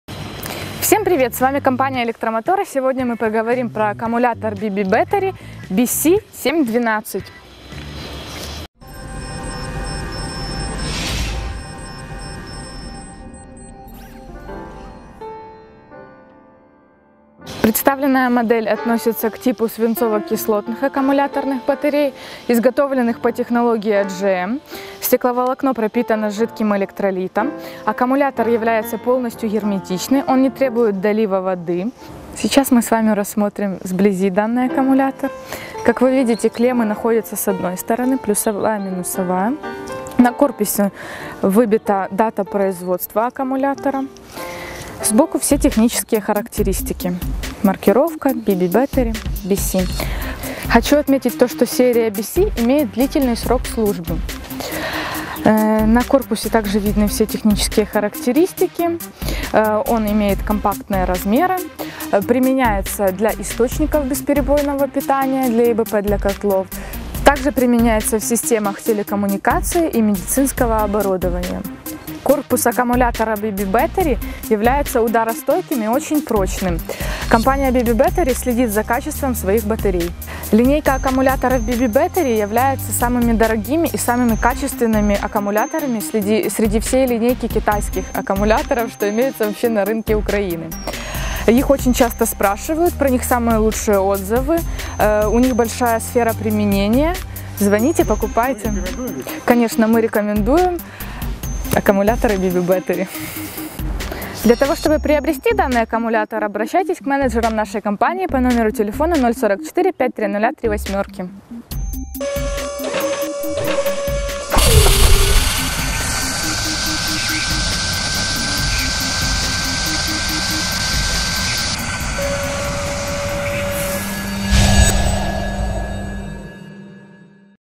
Аккумулятор B.B. Battery Bc 7-12
Аккумулятор B.B. Battery Bc 7-12  оптовая цена со склада .
Аккумуляторная батарея B.B. Battery BC 7-12 (12V 7Ah)
основные характеристики:
Емкость:7Ач
Напряжение составляет: 12Вольт
  Тип аккумулятора: Серия BC
Гарантия на аккумулятор:12 месяцев
Компания Электромотор Киев. Контактный телефон 5000888